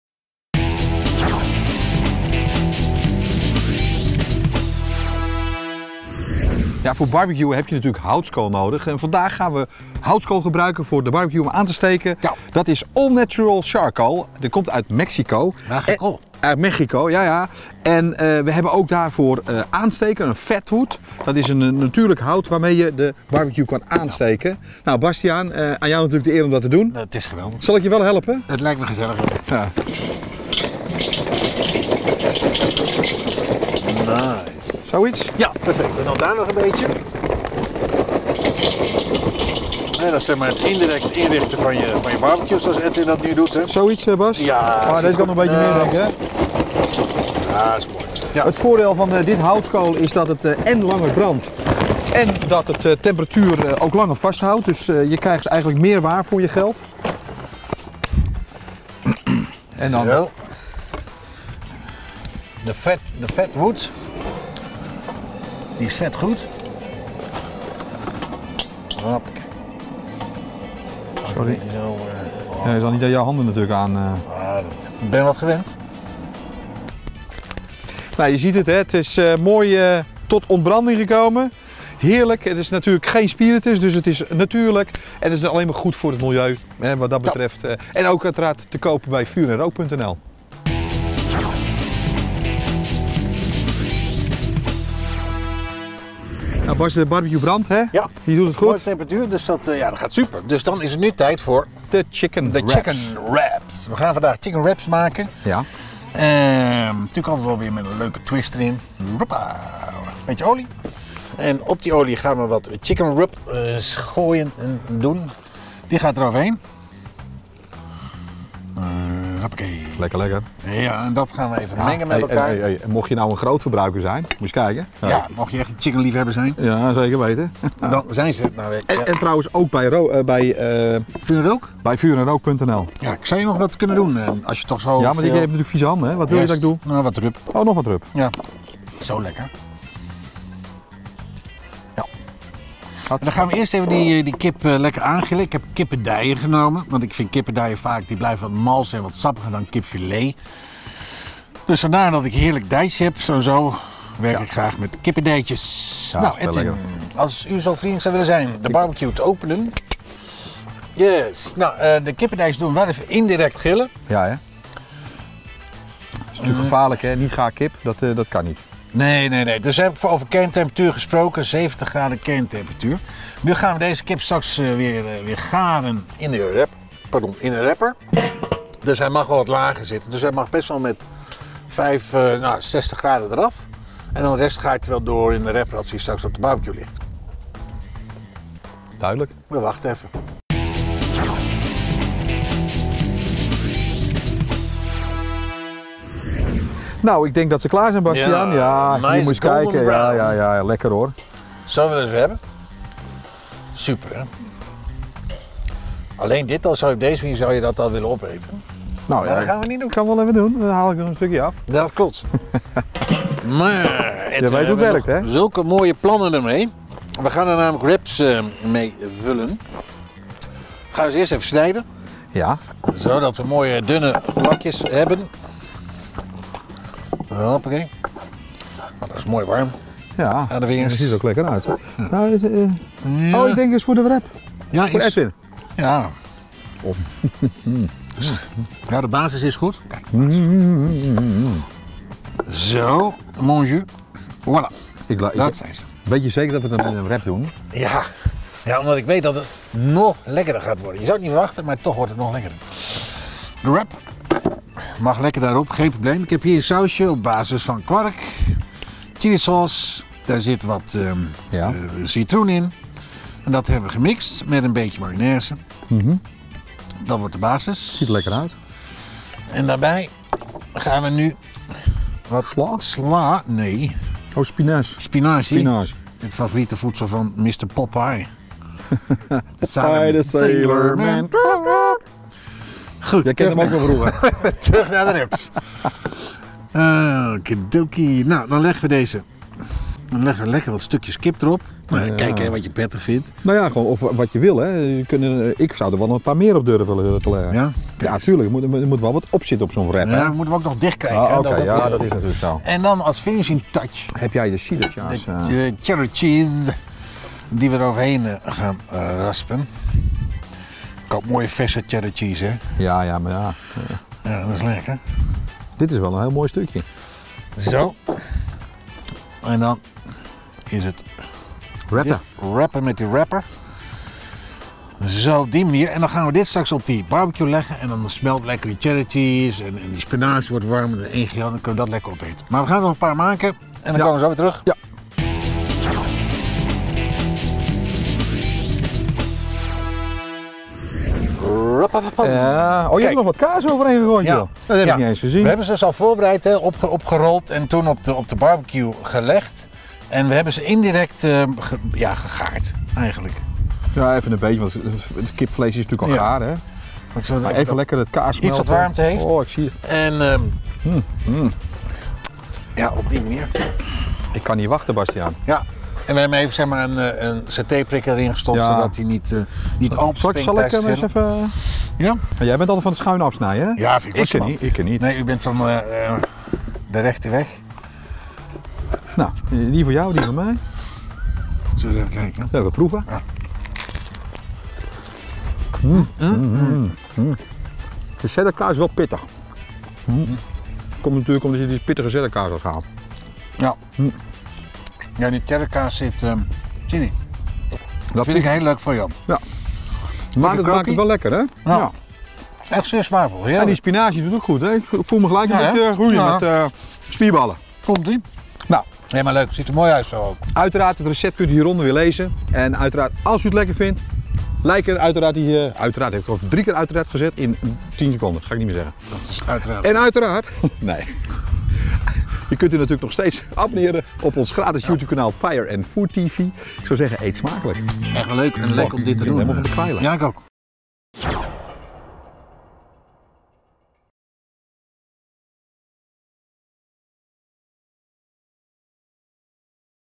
BBQ Chicken Wrap | Fire&Food TV
It's a wrap met dit gerecht! Gebruik voor dit recept kippendijen, want dit is veel sappiger en malser dan kipfilet. Lekker grillen, rollen, garen en eten maar!

Houd jij net als wij van grillen & barbecueën?
En grill met ons 365 dagen per jaar mee.